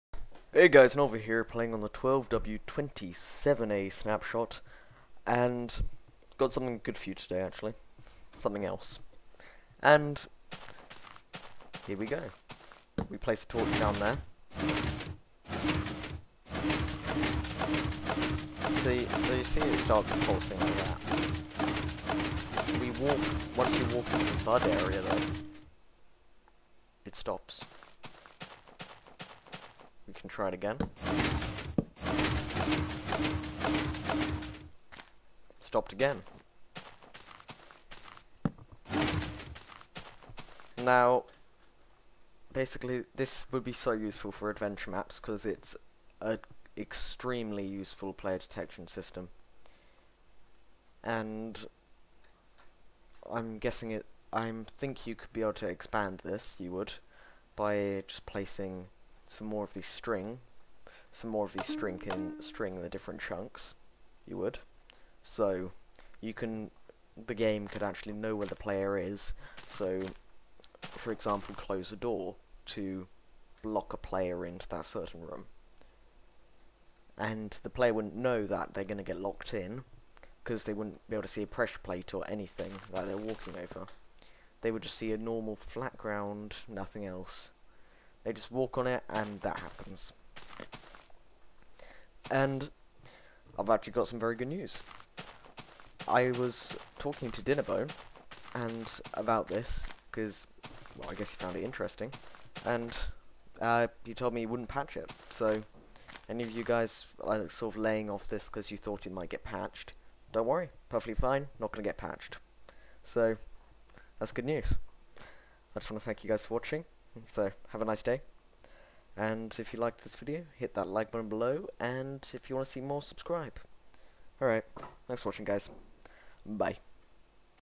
Player detection system
My new player detection system, extremely accurate and easy to build with an off/on switch as well :D
Thanks for clicking and have a nice day :D